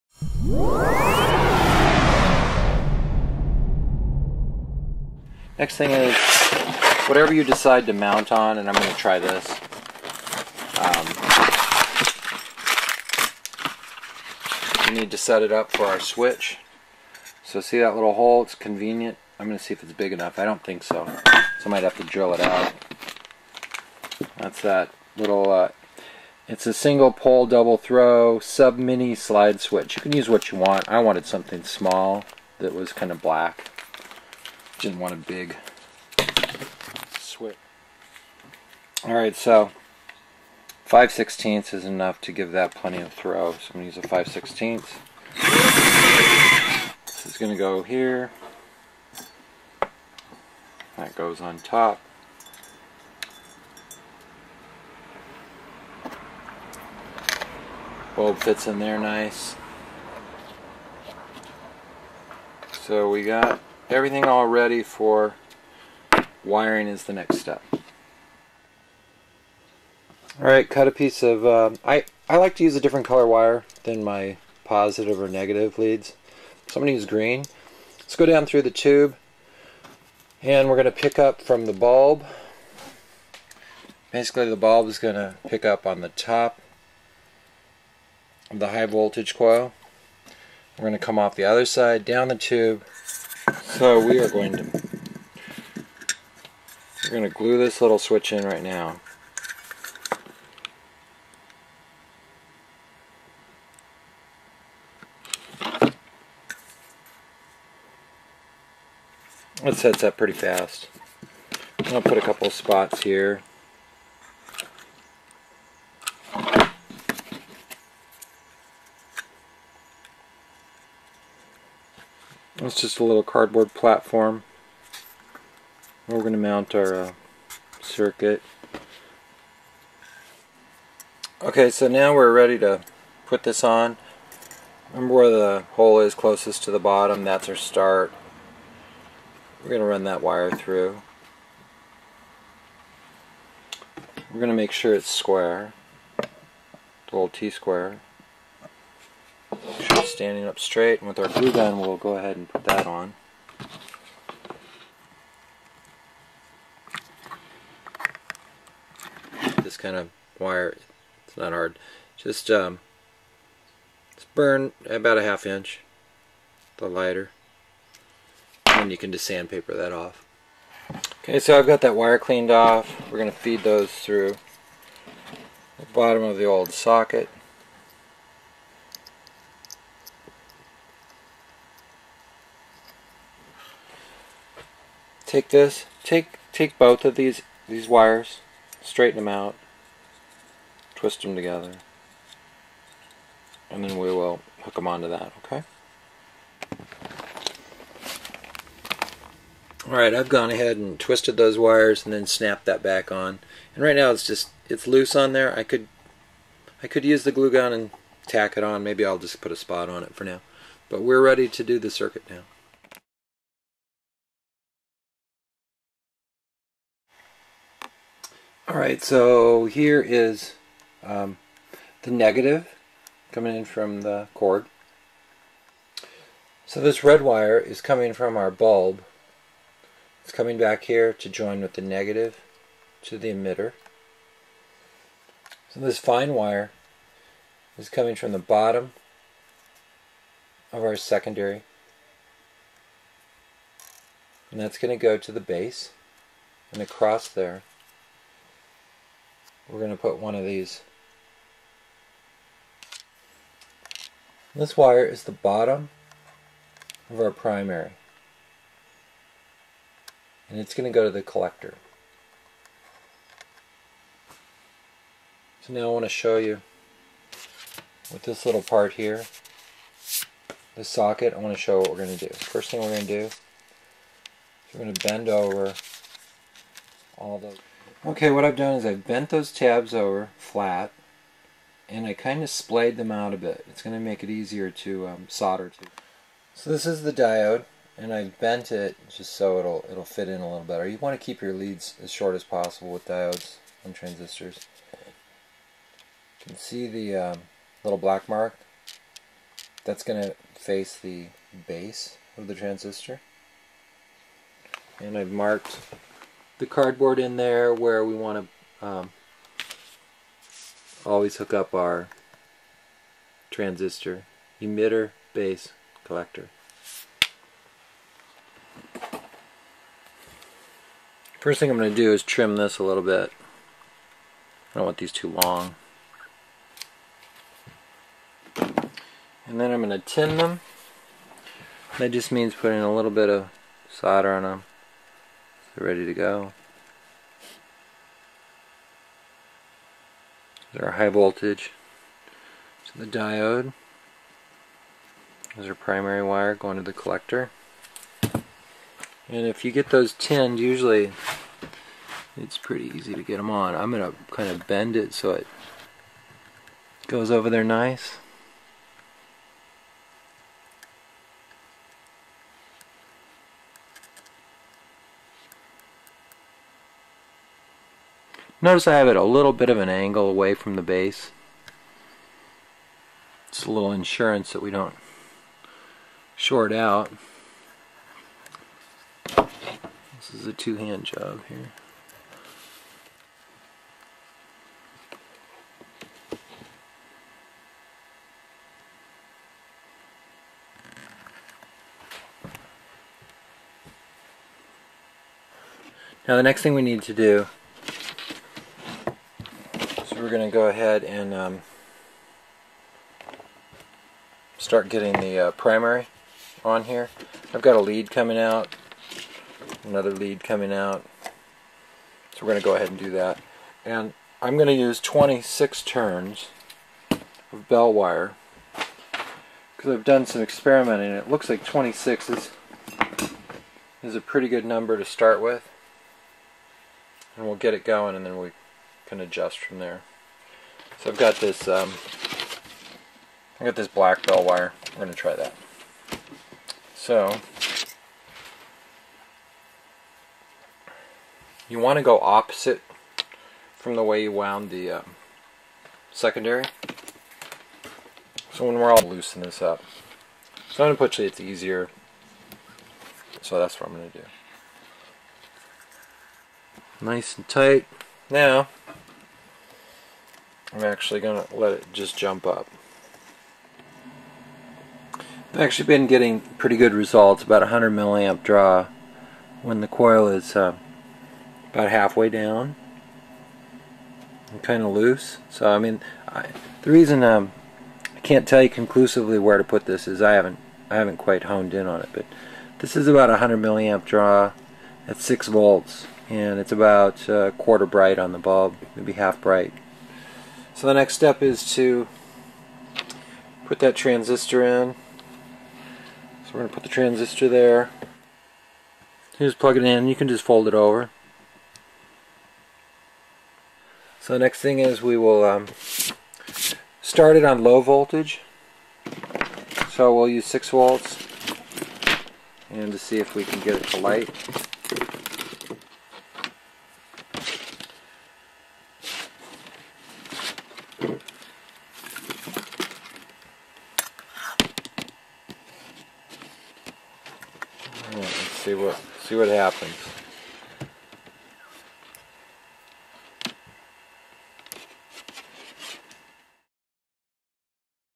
Lynx Joule Lamp - Part 2
Update:  I am changing the specified transistor from the 2N2222 class to a 2N3055 power transistor.  You don't need the socket now.  This transistor increases power available and is easier to install.  Or you can change the transistor after you have tried the 2N2222 - the wiring is the same.
Here is the "How to make" video series Part 2 on the Lynx Joule Lamp.
Note that the primary and secondary coils are wound in opposite directions.
In part 3 we will look at adjustments, amp draw, and comparison to a similar output incandescent lamp.  Also we will look at the amp draw of this CFL on a 12/120 volt inverter.